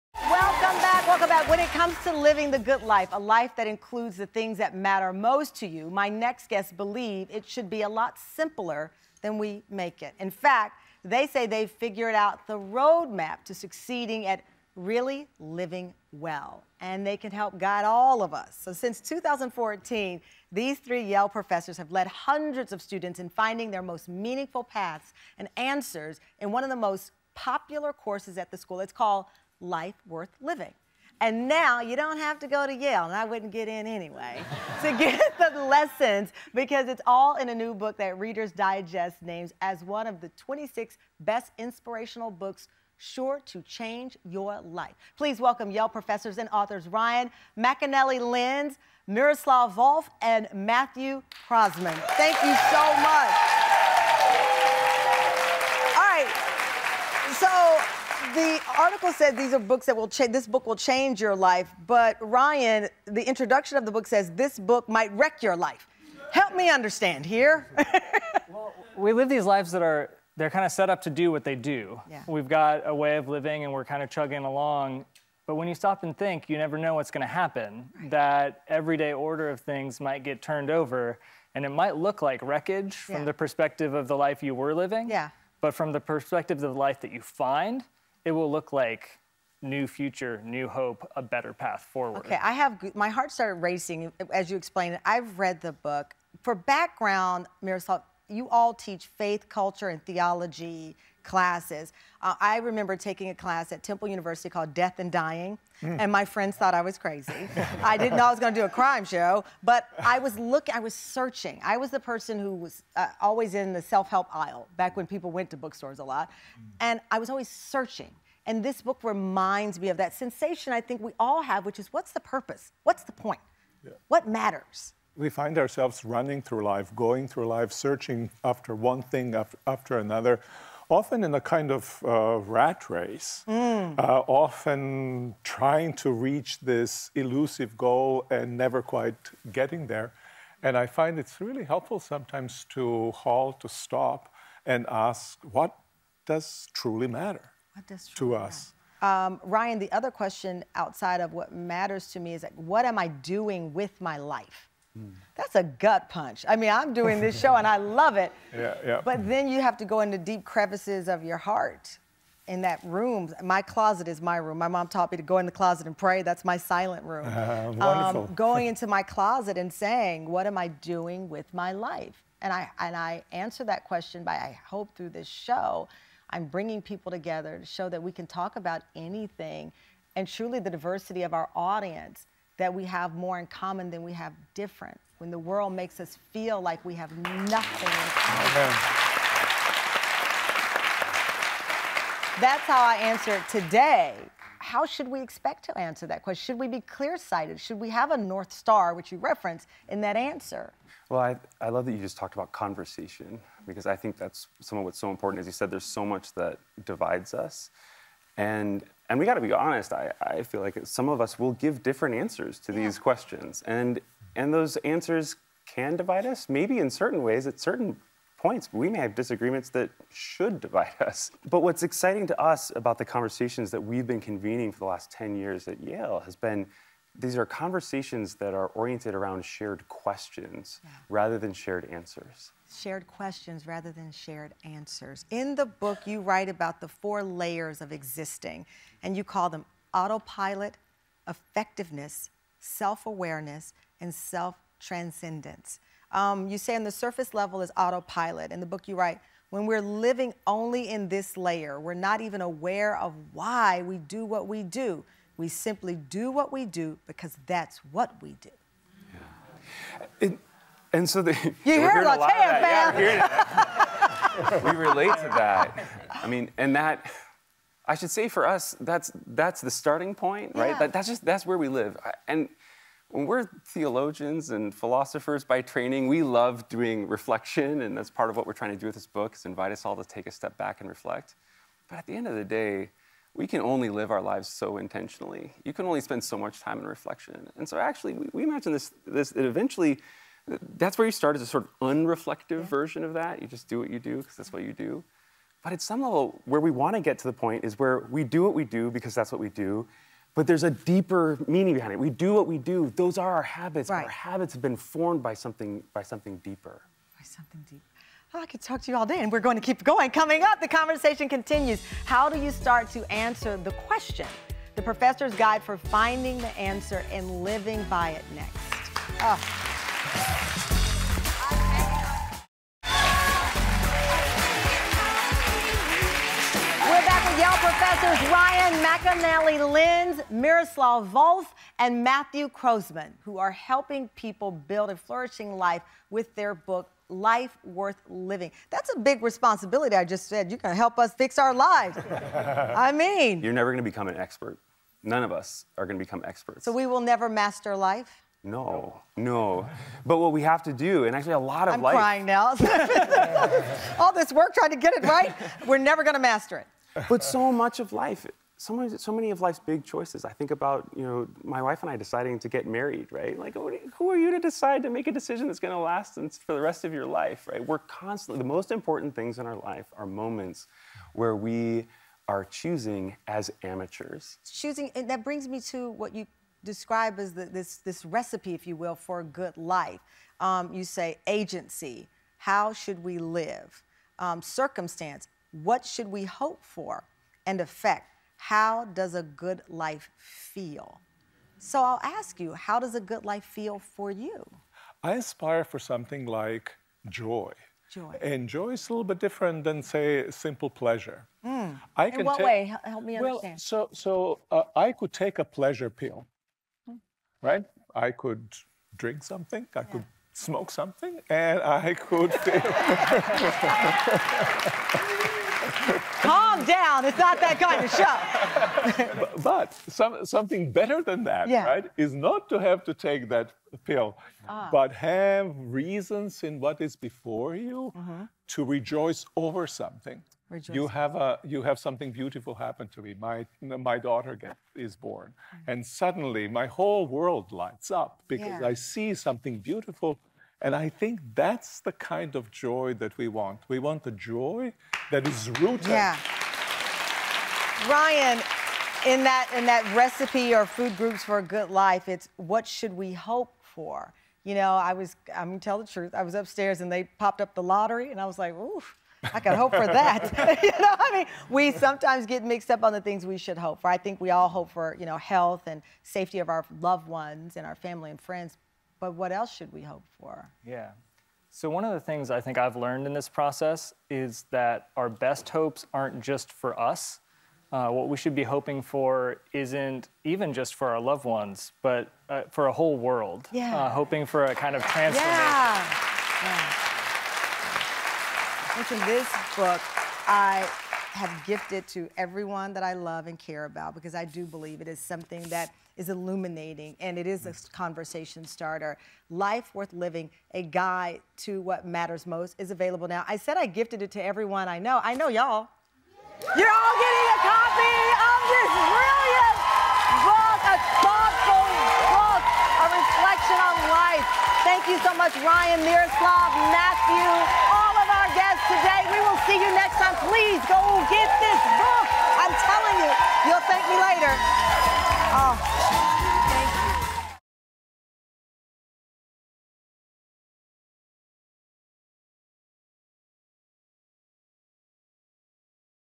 Three Yale Professors Are Teaching Us to Focus on What Truly Matters in New Book
Yale Professors Miroslav Volf, Matthew Croasmun, and Ryan McAnnally-Linz join our episode titled, “Popular and Positive” to talk about their book, “Life Worth Living: A Guide to What Matters Most.” This is a powerful sit-down about the power of purpose that you don’t want to miss!